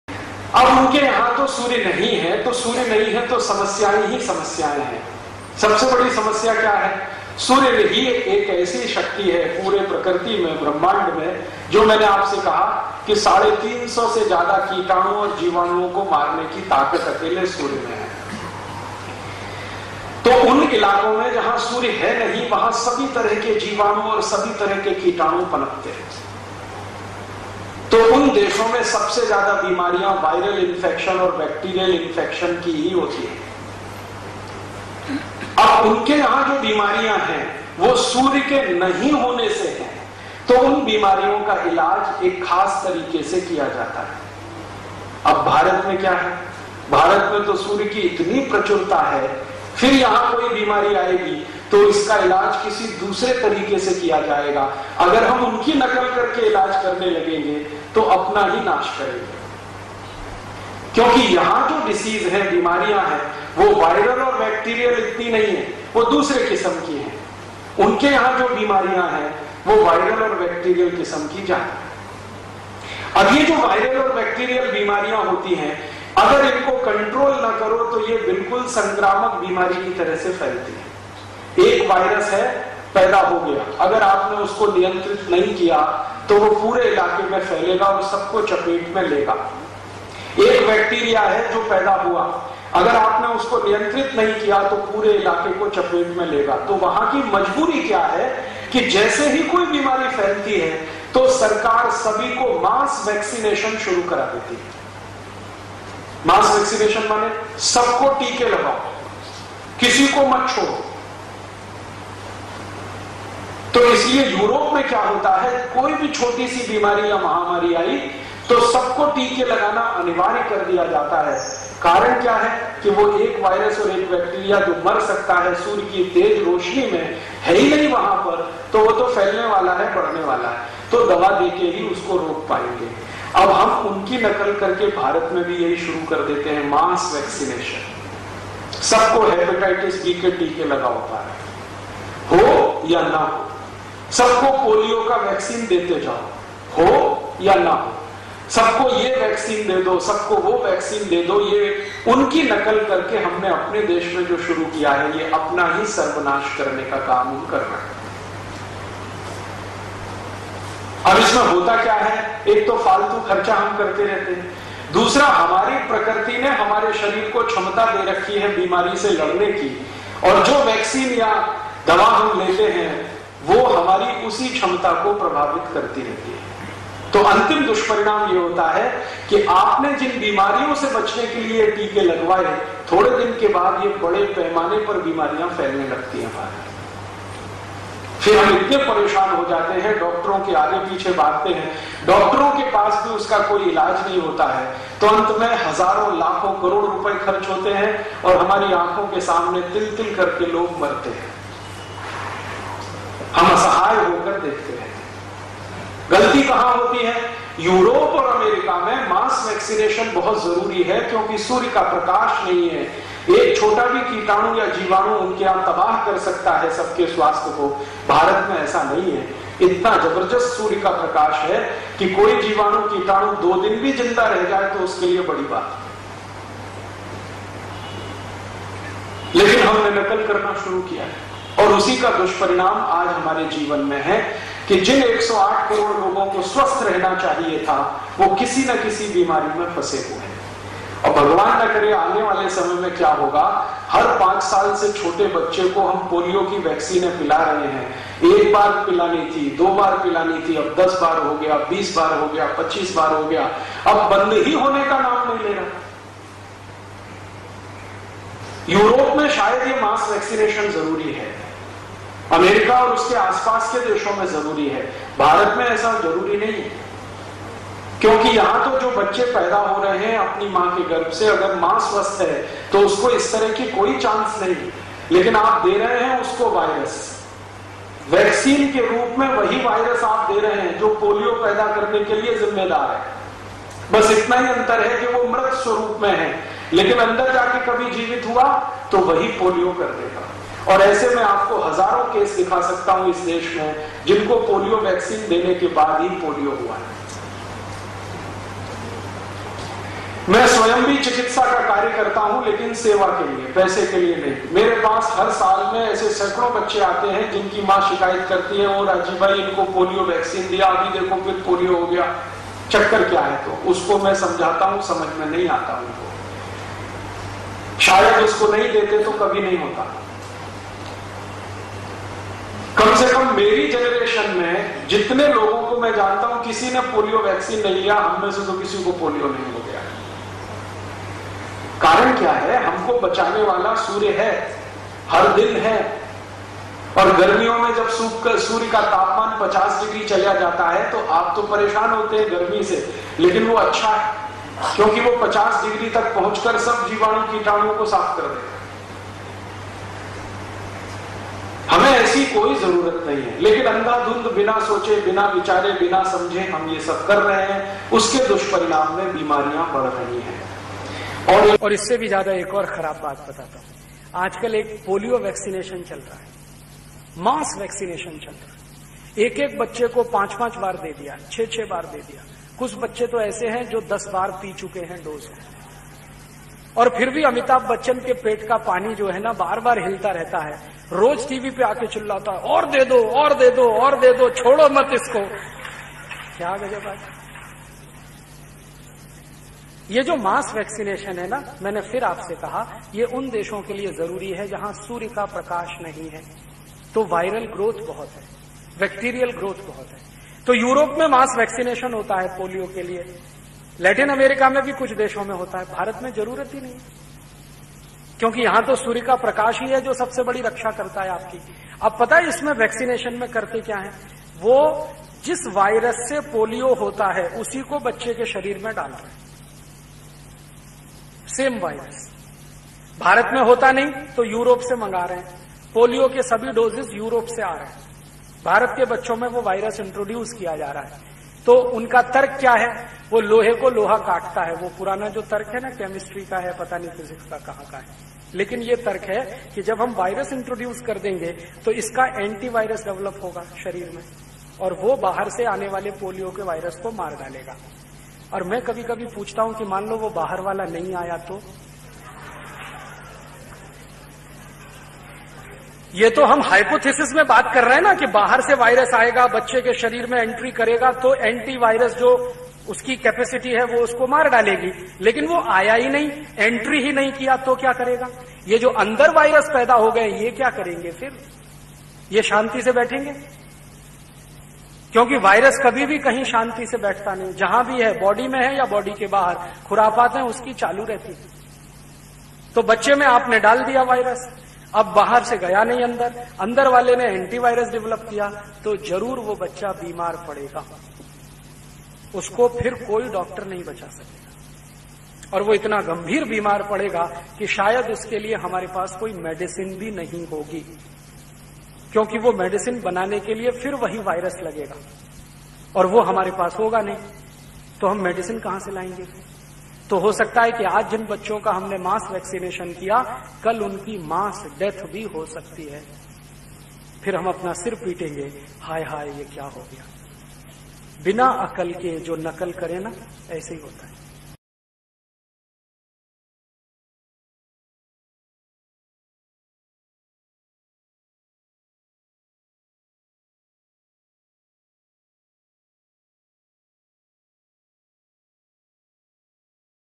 Truth-Rajiv Dixit on Vaccination in India
Rajiv Dixit explained the truth of virus Vaccination all over the world.